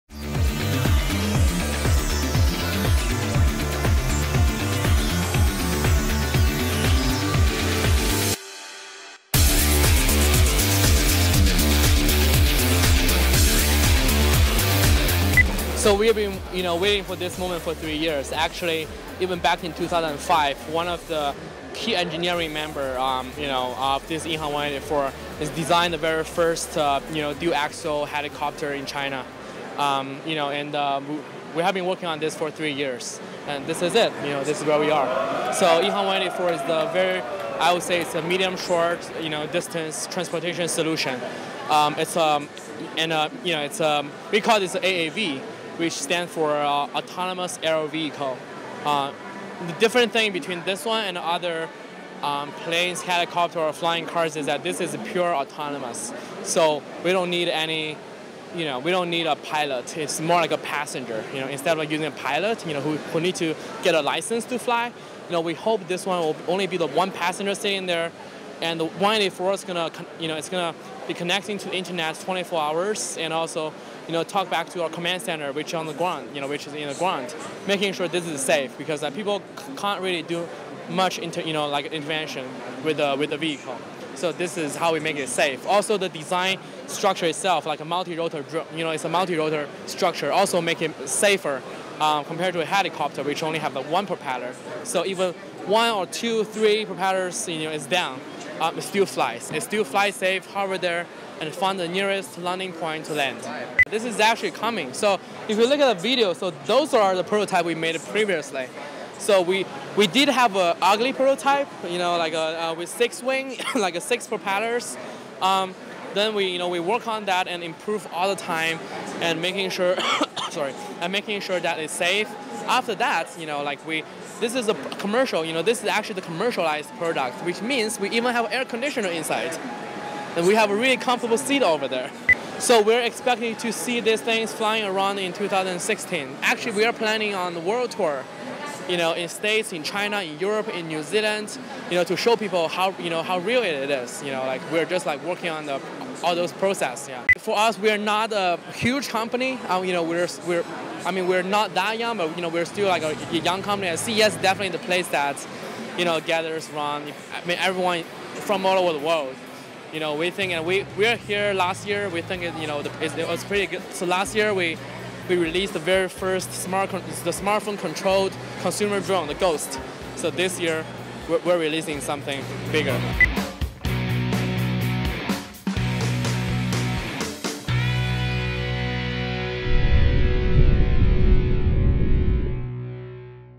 Behold - The Autonomous Aerial VEHICLE (EHang 184)
EHang, a Chinese unmanned aerial vehicle (UAV) company, unveiled at CES the EHang 184, the world’s first electric, personal Autonomous Aerial Vehicle (AAV) that will achieve humanity’s long-standing dream of easy, everyday flight for short-to-medium distances. Developed independently by EHang, the fully ready-to-fly AAV is a manned drone capable of automatically carrying a passenger through the air, simply by entering a destination into its accompanying smartphone app. Due to the 184’s fully automated navigation, made possible by EHang’s 24/7, real-time flight command center, passengers have no need for a pilot’s license – they simply sit back and let the drone take over from there. This first realization of automated flight signifies a major turning point not only for the transportation industry, but also for a huge swath of other fields such as shipping, medical care and retail.